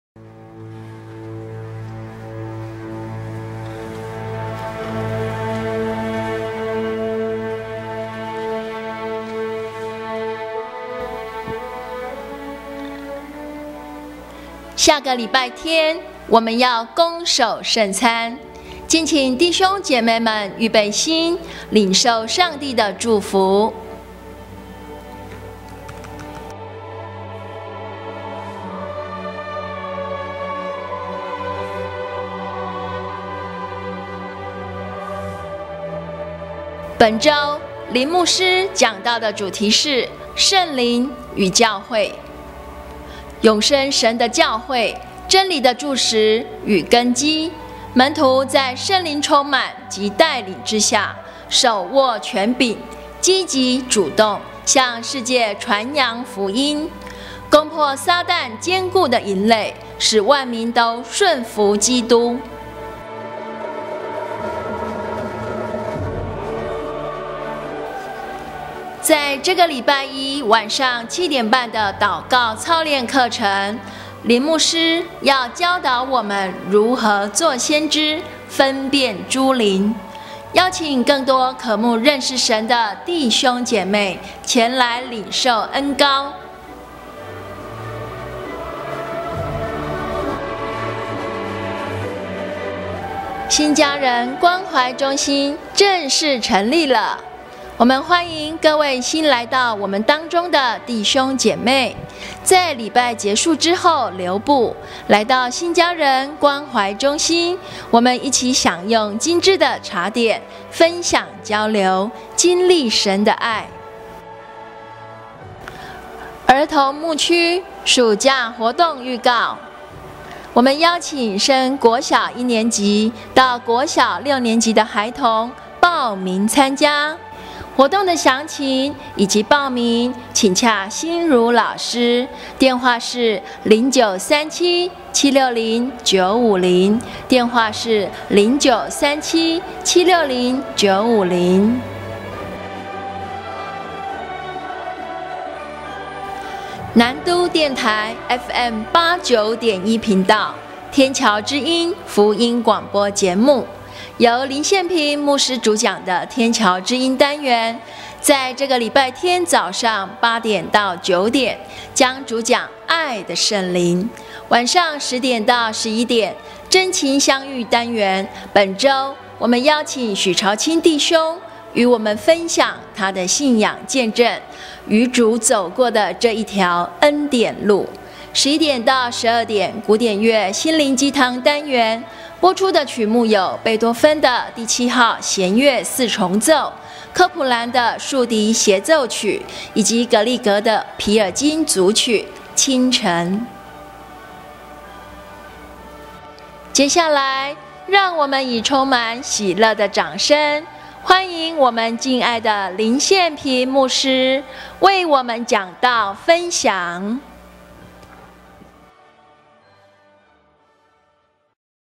天橋教會2016第21週報消息5/22-5/28 主播 / 謝佳蓉 3m31s
1.下個禮拜天5/29恭守聖餐，敬請弟兄姐妹們預備心，領受上帝的祝福。
2.本21週，林憲平牧師講題：聖靈與教會 (背景畫面 : The Mission 教會1986)
3. 在禮拜一晚上7:30，禱告操練課程，本週教導我們「如何作先知，分辨諸靈」
4. 兒童牧區暑假活動預告1.天橋兒童冠冕理財營，時間：7/11-15(週一～週五）上午9:00-下午4:00地點：天橋教會2.天橋兒童歡樂夏令營，時間：7/18-22(週一～週五）晚上7:00-9:00地點：天橋教會活動詳請及報名請洽欣如老師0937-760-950或Line ID: shinrukao